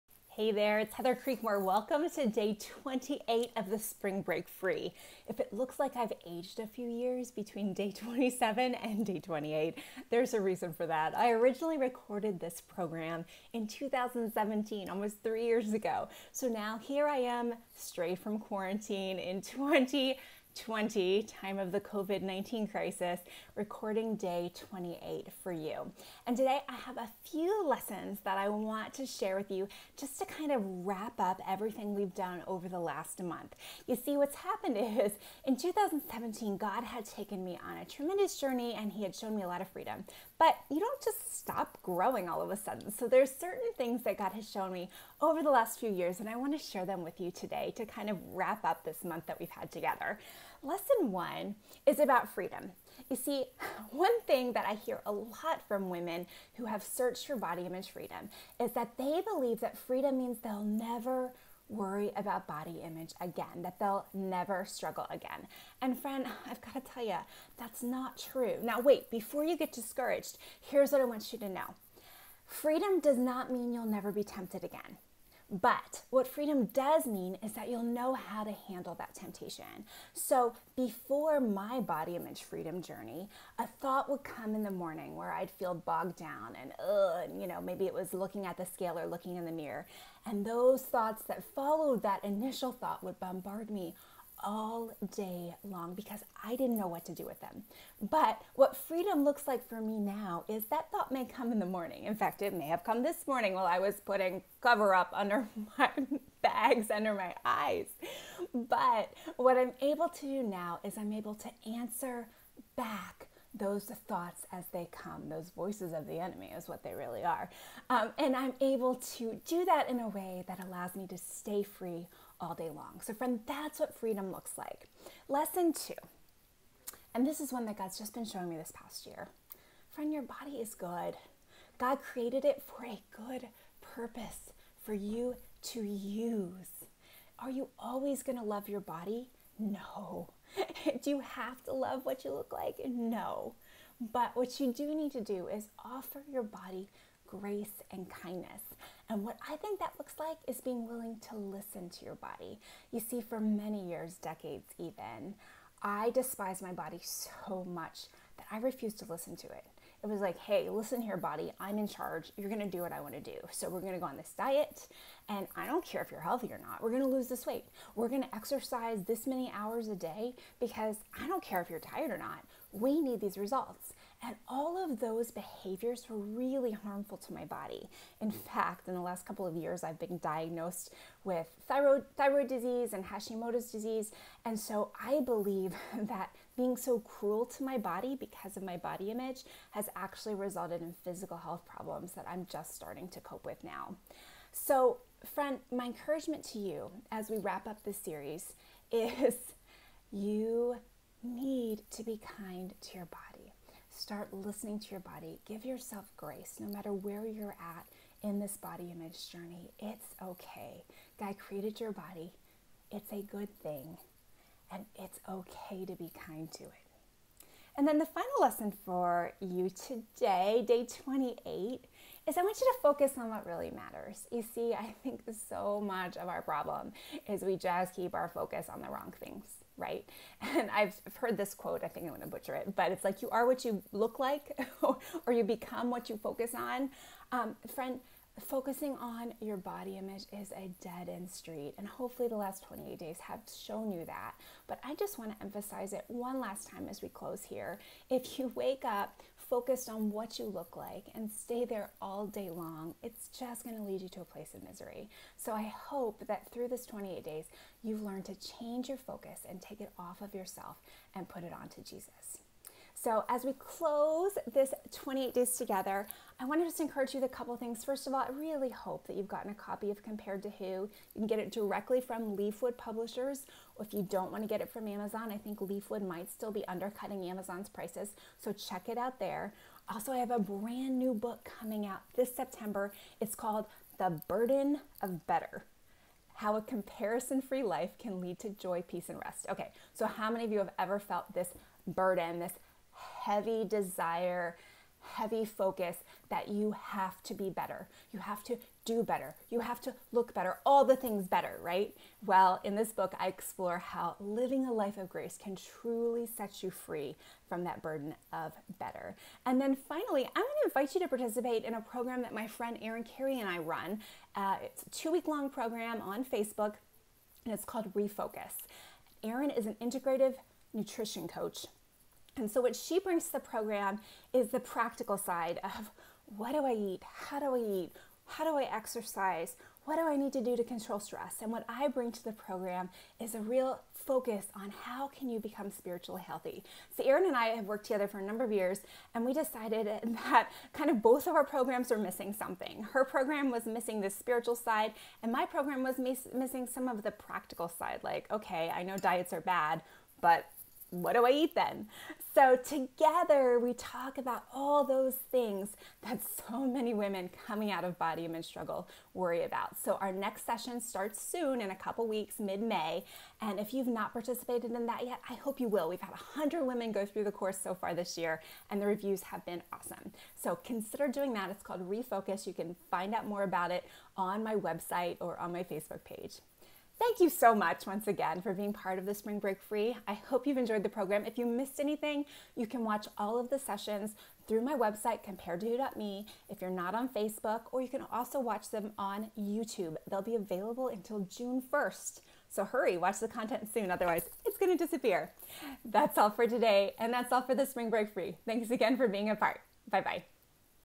Spring Break Free: Body Image Freedom Course! Day 28 -- 3 Lessons as we close!
This 28 Day Body Image Freedom Course is free for a limited time. Follow along through the month of April and find the root of your body image issues and encouragement as to how to find body image freedom through the Gospel of Jesus Christ!